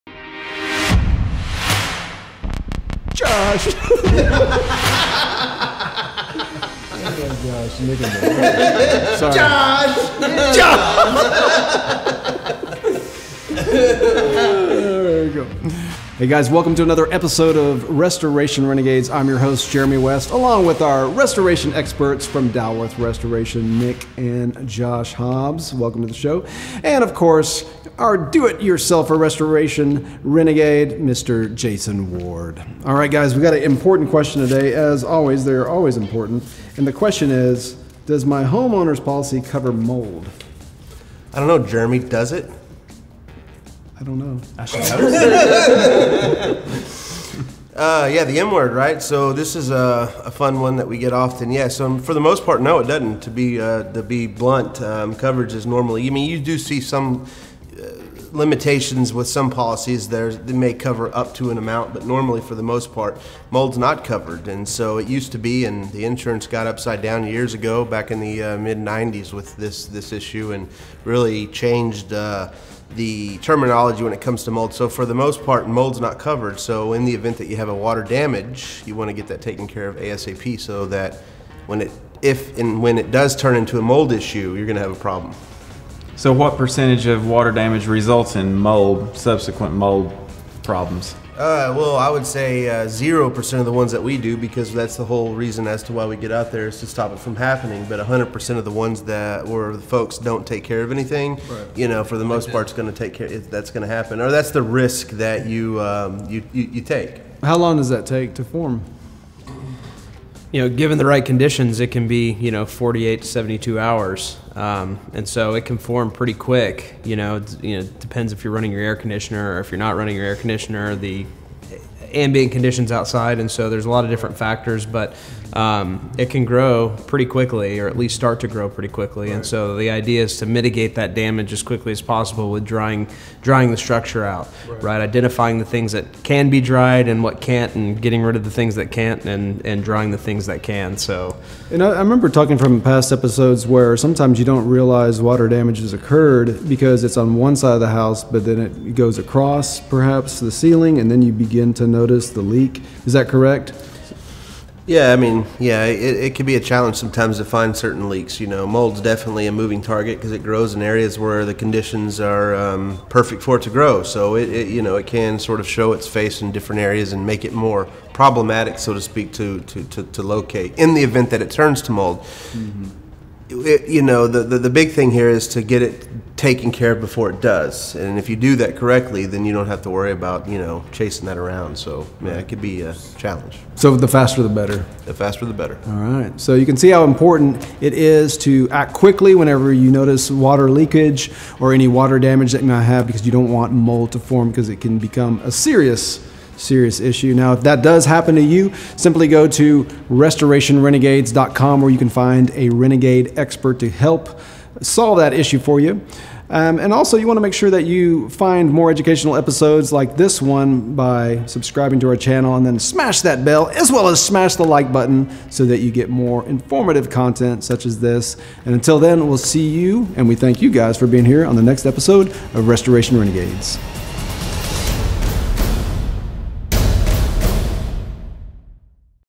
Does my Homeowners policy cover mold damage? - Restoration Renegades #8
Mold damage homeowner policies can be tough to navigate. Get some advice from the Restoration pros.